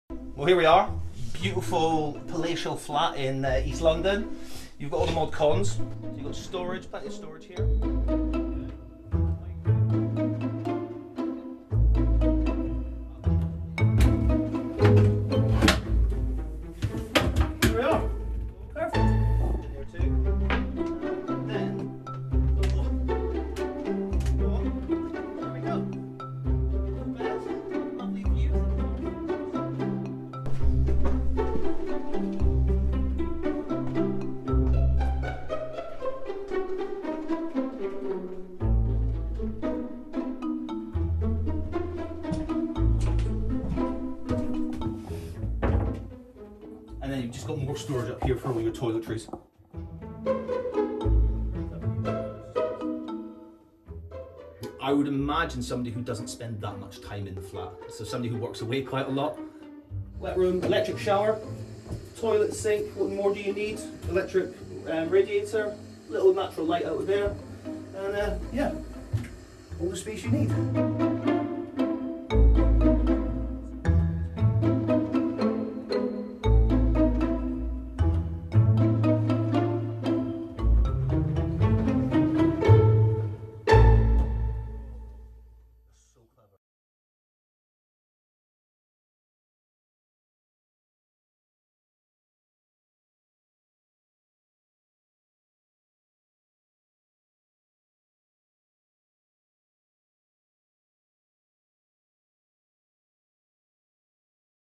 Would you buy "London's tiniest flat" for £90K?
The smallest flat in London has sold at auction for £90,000. The 75 sq ft (7 sq m) property in Clapton only boasts a single bed, storage and a microwave. A separate "wet-room" has a toilet, sink, shower and towel rail.

A German investor bought the east London flat at auction last month. The bijou dwelling is an example of what campaigners say is a growing phenomenon of micro-homes, driven by high rent and property prices.

Video by Sam Francis